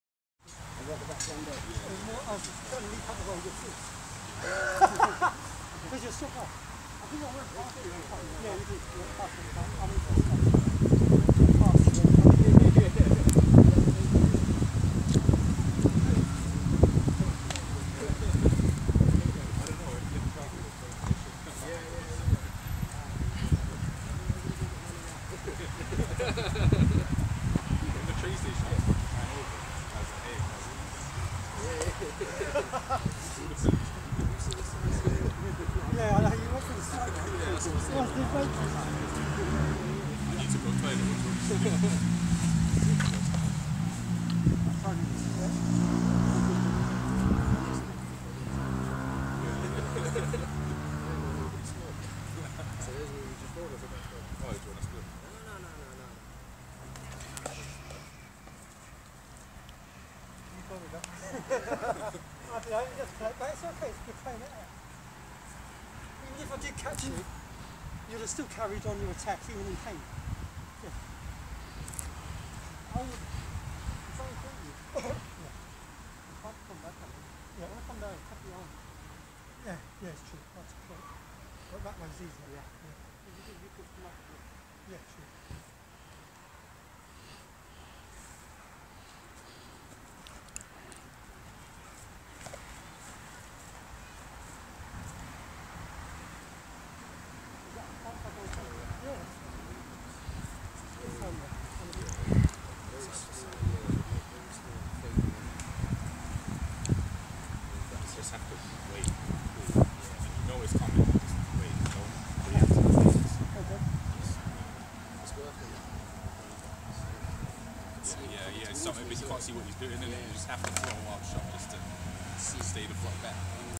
Knife Sparring (1)
Me (Wing Chun) knife sparring with Wu Sifu  (Sword Craft) of WuShan for the 1st time..

This was a friendly exchange..both of us using the knife..a weapon that is not part of our respective systems that we practise...

This was a pressure test to see if my Kung fu skills are transferable...

Sifu Mohammed Akhtar, plz see FB "Bedfordshire Wing Chun Kuen" or check